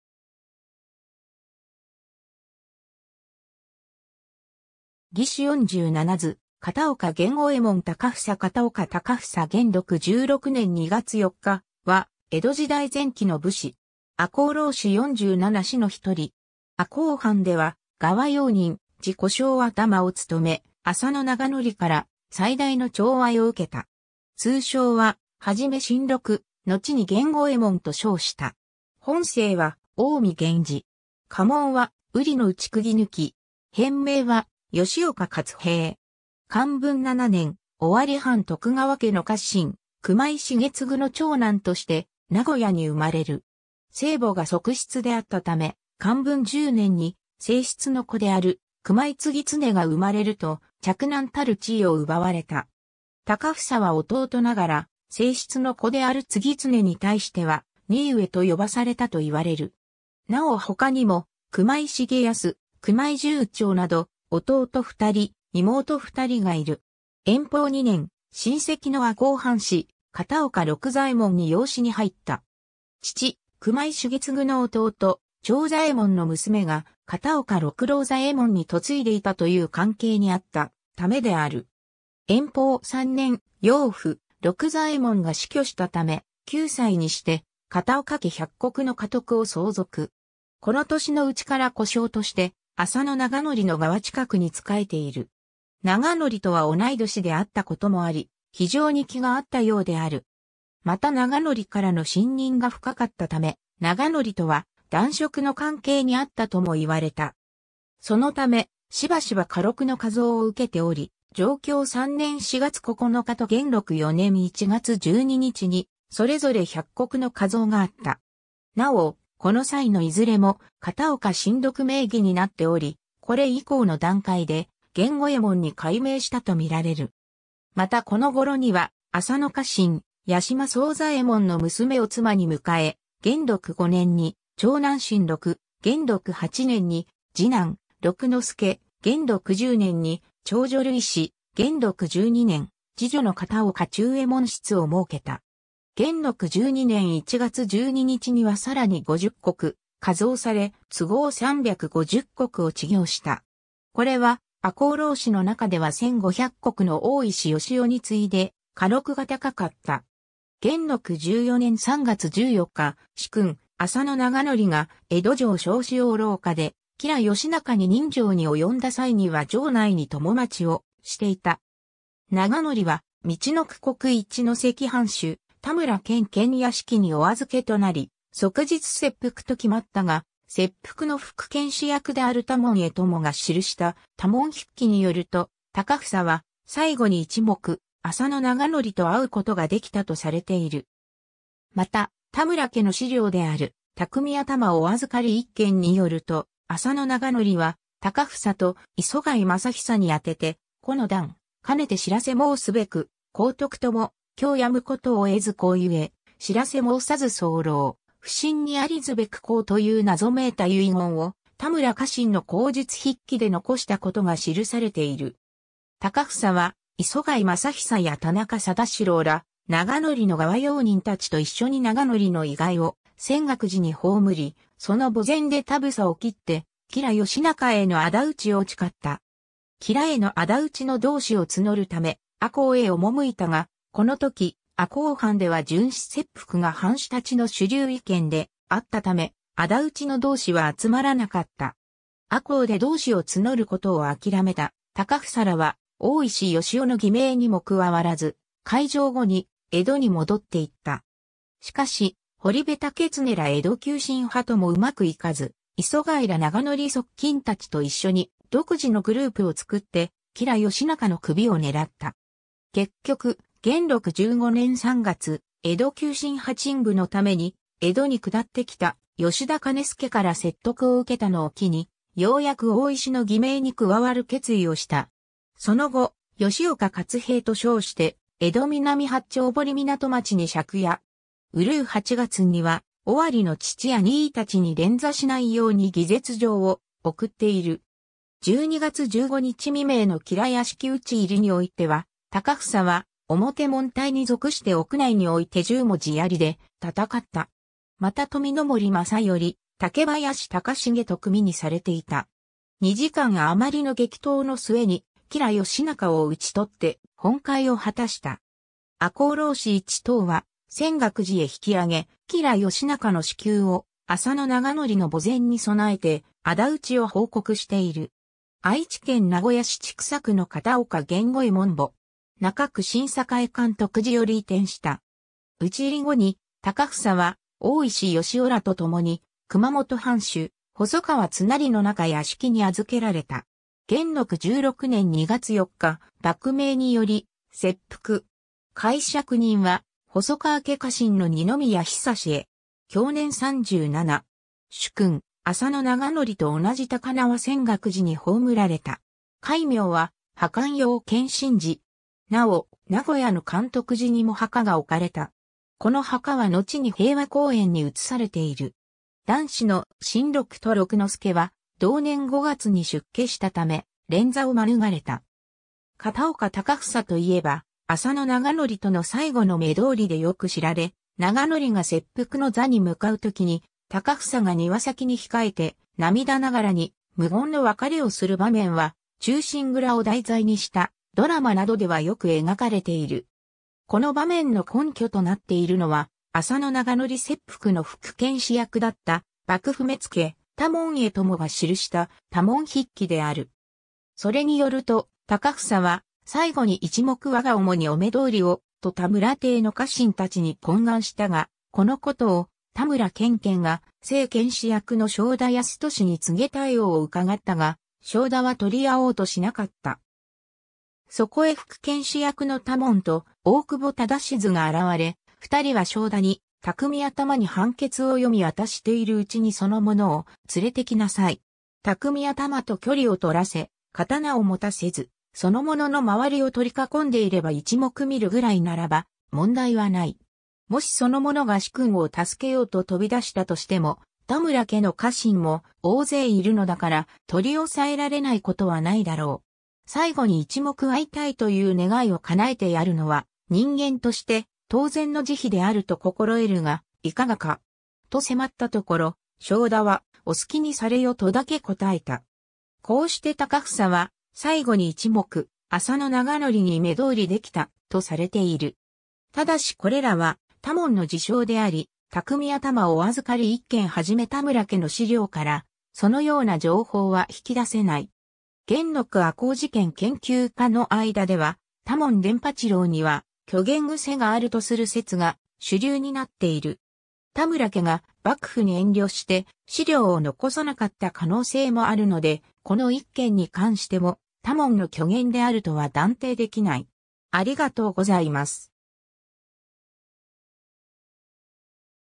片岡高房
『義士四十七図　片岡源五右衛門高房』（尾形月耕画） 片岡 高房（かたおか たかふさ　寛文7年（1667年） - 元禄16年2月4日（1703年3月20日））は、江戸時代前期の武士。
赤穂浪士四十七士の一人。
赤穂藩では側用人・児小姓頭をつとめ、浅野長矩から最大の寵愛を受けた。
通称は、はじめ新六（しんろく）、のちに源五右衛門（げんごえもん）と称した。
本姓は近江源氏。
家紋は瓜の内釘貫。
変名は、吉岡勝兵衛。
 寛文7年（1667年）、尾張藩徳川家の家臣・熊井重次（知行300石御蔵米120石）の長男として名古屋に生まれる。
生母が側室であったため、寛文10年（1670年）に正室の子である熊井次常が生まれると、嫡男たる地位を奪われた。
高房は弟ながら正室の子である次常に対しては「兄上」と呼ばされたといわれる。
なお他にも熊井重康、熊井重長など弟2人、妹2人がいる。
 延宝2年（1674年）、親戚の赤穂藩士・片岡六左衛門（知行100石）に養子に入った。
父・熊井重次の弟・長左衛門の娘が片岡六郎左衛門に嫁いでいたという関係にあったためである。
 延宝3年（1675年）、養父・六左衛門が死去したため、9歳にして片岡家100石の家督を相続。
この年のうちから小姓として浅野長矩の側近くに仕えている。
長矩とは同い年であったこともあり、非常に気が合ったようである。
また長矩からの信任が深かったため、長矩とは男色の関係にあったともいわれた。
 そのため、しばしば家禄の加増を受けており、貞享3年（1686年）4月9日と元禄4年（1691年）1月12日にそれぞれ100石の加増があった。
なお、この際のいずれも「片岡新六」名義になっており、これ以降の段階で源五右衛門に改名したとみられる。
またこの頃には浅野家臣・八嶋惣左衛門の娘を妻に迎え、元禄5年（1692年）に長男新六、元禄8年（1695年）に次男六之助、元禄10年（1697年）に長女るい子（津金善次郎室）、元禄12年（1699年）、次女の片岡中右衛門室を儲けた。
元禄12年（1699年）1月12日にはさらに50石加増され、都合350石を知行した。
これは赤穂浪士の中では1500石の大石良雄に次いで家禄が高かった。
 元禄14年（1701年）3月14日、主君・浅野長矩が江戸城松之大廊下で吉良義央に刃傷に及んだ際には城内に供待ちをしていた。
長矩は陸奥国一関藩主田村建顕屋敷にお預けとなり、即日切腹と決まったが、切腹の副検死役である多門重共（幕府目付）が記した『多門筆記』によると、高房は最期に一目浅野長矩と会うことができたとされている。
また田村家の資料である『内匠頭お預かり一件』によると、浅野長矩は高房と礒貝正久に宛てて「孤の段、兼ねて知らせ申すべく候得共、今日やむ事を得ず候故、知らせ申さず候、不審に存ず可く候」という謎めいた遺言を田村家臣の口述筆記で残したことが記されている（ただし、文章がしり切れてしまっており不自然な内容であるため、この後に続く文は江戸幕府を憚って田村家で消された可能性が高い）。
 高房は礒貝正久や田中貞四郎ら長矩の側用人たちと一緒に長矩の遺骸を泉岳寺に葬り、その墓前で髻を切って吉良義央への仇討ちを誓った。
吉良への仇討ちの同志を募るため、赤穂へ赴いたが、このとき赤穂藩では殉死切腹が藩士達の主流意見であったため、仇討ちの同志は集まらなかった。
赤穂で同志を募ることを諦めた高房らは、大石良雄の義盟にも加わらず、開城後に江戸に戻っていった。
しかし、堀部武庸ら江戸急進派ともうまくいかず、礒貝ら長矩側近たちと一緒に独自のグループをつくって、吉良義央の首を狙った。
結局、元禄15年（1702年）3月、江戸急進派鎮撫のために江戸に下ってきた吉田兼亮から説得を受けたのを機に、ようやく大石の義盟に加わる決意をした。
その後、吉岡勝兵衛と称して江戸南八丁堀湊町に借家。
閏8月には尾張の父や兄（本当は弟だが）達に連座しないように義絶状を送っている。
 12月15日未明の吉良屋敷討ち入りにおいては、高房は表門隊に属して屋内において十文字槍で戦った。
また富森正因・武林隆重と組にされていた（山鹿流兵法に基づいて三人一組の編成になっていた）。
2時間あまりの激闘の末に、吉良義央を討ち取って本懐を果たした。
赤穂浪士一党は泉岳寺へ引き上げ、吉良義央の首級を浅野長矩の墓前に供えて仇討ちを報告している。
 愛知県名古屋市千種区の片岡源五右衛門墓。
中区新栄乾徳寺より移転した。
 討ち入り後に、高...